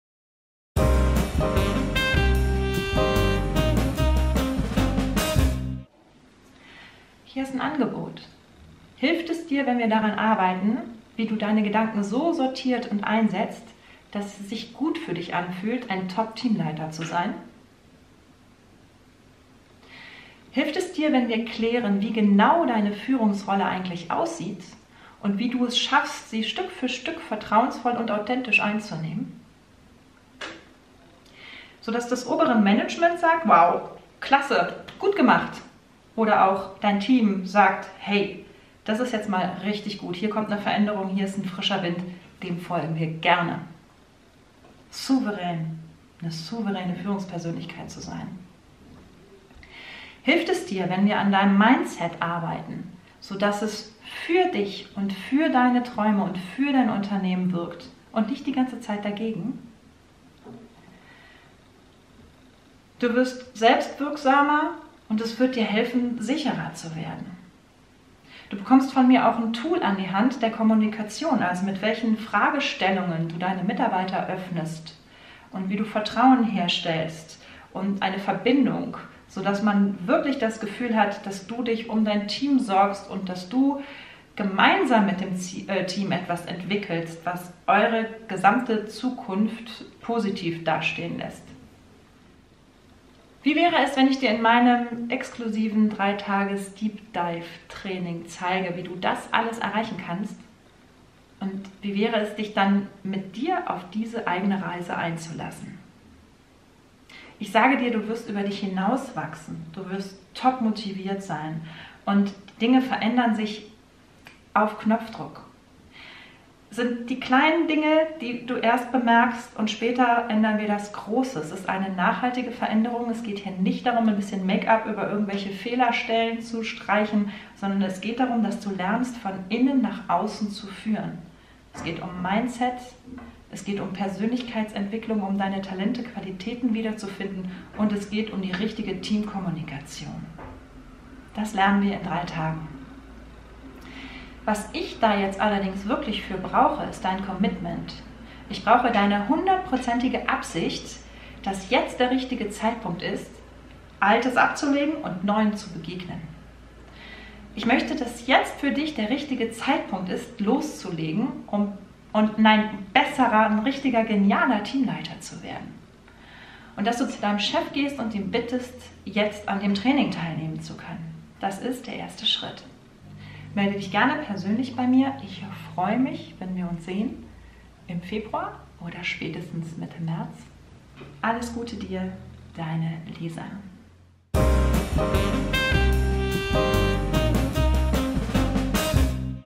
Vom Kollegen zum Chef – Dein exklusives, 3-tägiges deep dive Training für junge Führungskräfte
Schluss mit Ängsten.
Schluss mit Unsicherheit.
Schluss mit scheinbar unfähigen Führungskräften.

Mit diesem 3-tägigen, exklusiven deep dive-Training gelingt Dir oder Deinem frischen Führungsteam der Aufstieg in die neue Führungsrolle. Garantiert.

Denn es ist hocheffizient, wirksam und nachhaltig.
Atme endlich auf, überzeuge mit Ideen, sei entspannt beim nächsten Mitarbeitergespräch, weil Du Dich traust, Deine neue Rolle als wirkungsvolle, vertrauensvolle und mitreissende Führungspersönlichkeit anzunehmen. Ohne bossy oder von oben herab zu wirken.
Ganz souverän. Ganz Du.
Und Dein Team? Das geht mit, ist sogar begeistert von Dir als Teamleiter. Versprochen!

Herzlich
Deine Lisa (seit 1995 leidenschaftliche Mitarbeiterentwicklerin)

Oder persönliche Email an Lisa:
lisa. boje (at) die-hotelharmonisierer .com